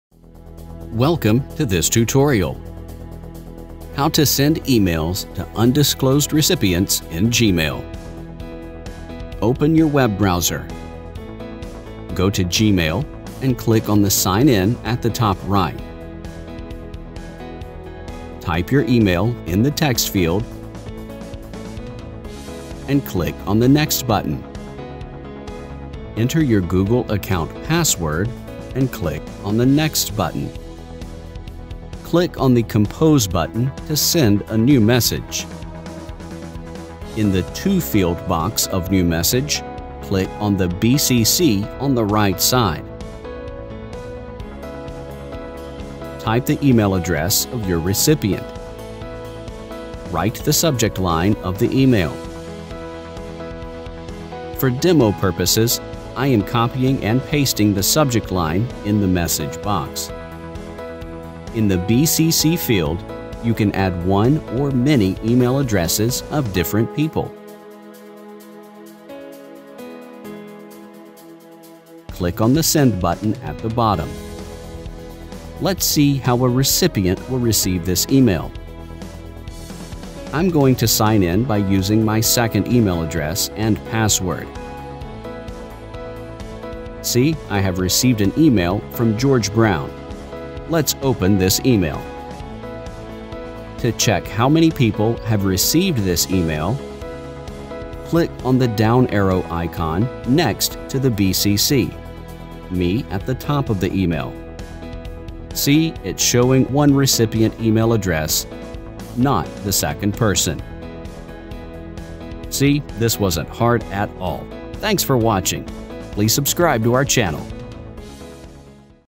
How to Send Emails to Undisclosed Recipients in Gmail
You want to circulate an email to many people but don’t want to disclose the other recipients. Here is the solution in this video. It is very easy to send an email and hide the email addresses of other recipients. Follow the steps as shown in this video.
1. Open your web browser.
2. Go to Gmail and click on the Sign in at the top right.
3. Type your email in the text field, and click on the Next button.
4. Enter your Google account password, and click on the Next button.
5. Click on the Compose button to send a new message.
6. In the To field box of New Message, click on the Bcc on the right-side.
8. Write the Subject line of the email.
9. For demo purposes, I am copying and pasting the Subject line in the message box.
10. In the Bcc filed, you can add one or many email addresses of different people.
11. Click on the Send button at the bottom.
12. Let’s see, how a recipient will receive this email.
14. See, I have received an email from George Brown.
15. Let’s open this email.
16. To check, how many people have received this email.
17. Click on the down arrow icon next to the bcc: me at the top of the email.